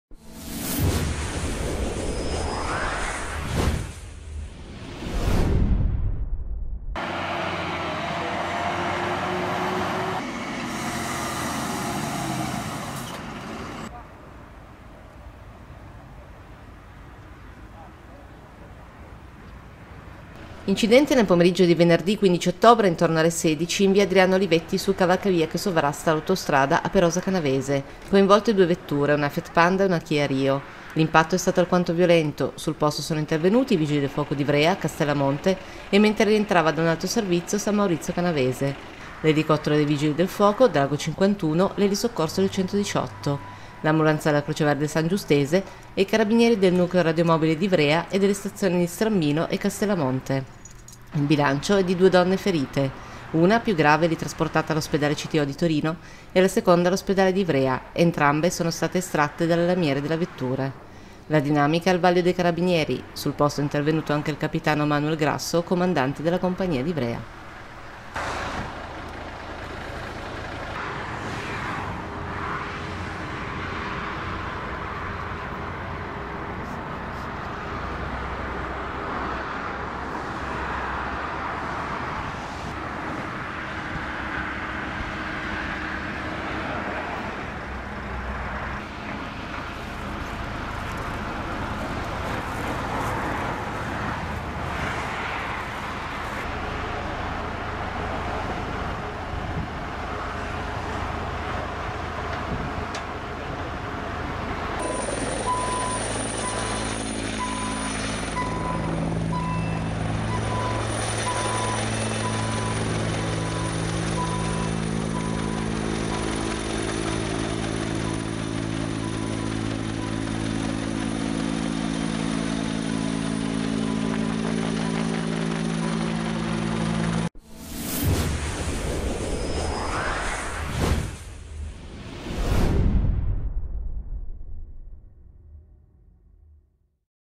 PEROSA CANAVESE - Scontro frontale tra due auto sul cavalcavia dell'autostrada
PEROSA CANAVESE - Scontro frontale tra due auto sul cavalcavia dell'autostrada, due donne ferite